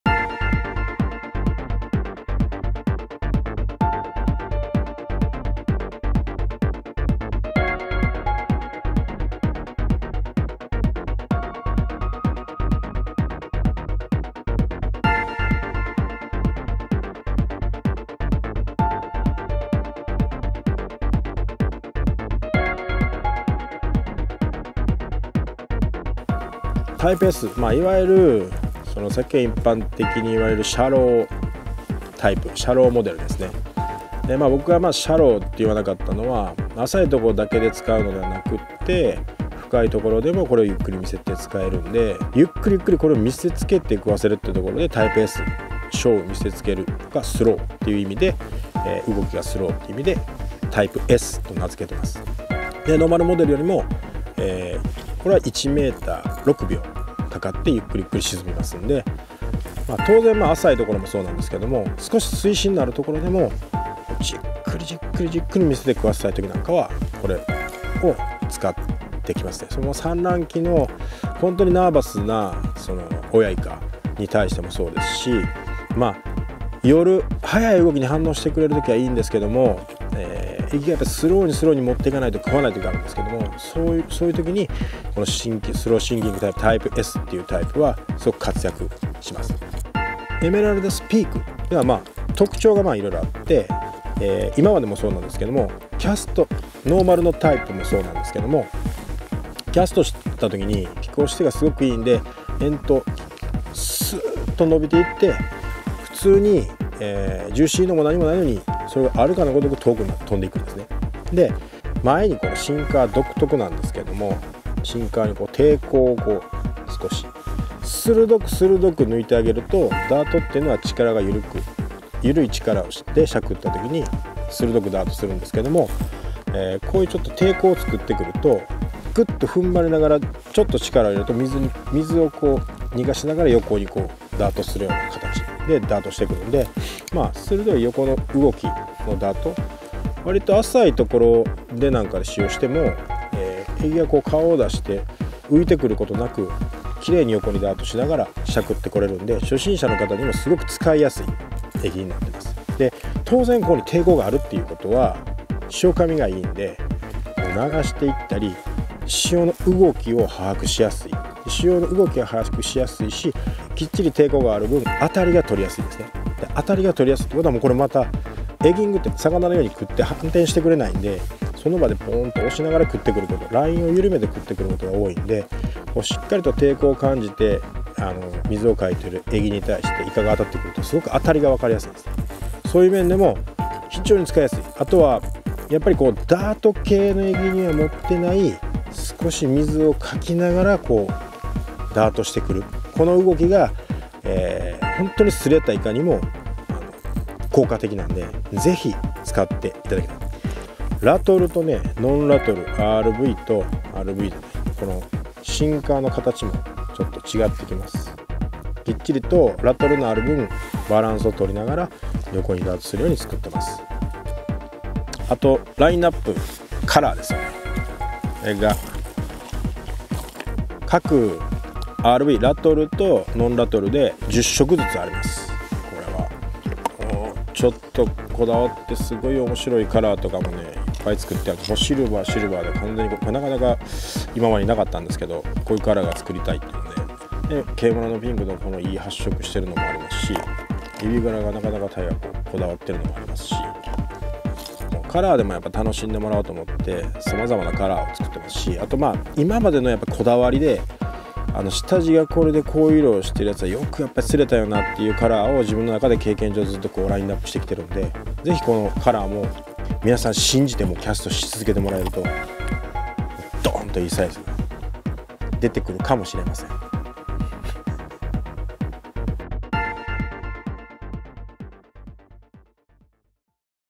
[エギング]衝撃ヒットシーン！春イカ攻略の切り札エメラルダスピークTypeS登場
タックル情報の確認は「もっと見る」をクリック
春の親イカシーズンの切り札となる新兵器エメラルダスピークTypeSを山田ヒロヒトが徹底解説！イカのチェイス、アタックからヒットまでを収めたヒットシーンも必見です。

■アングラー
山田ヒロヒト（ヤマラッピ）

■ロケ地　長崎県五島列島

■タックルデータ
ロッド：エメラルダスSTOIST　ST

# エギング#エメラルダス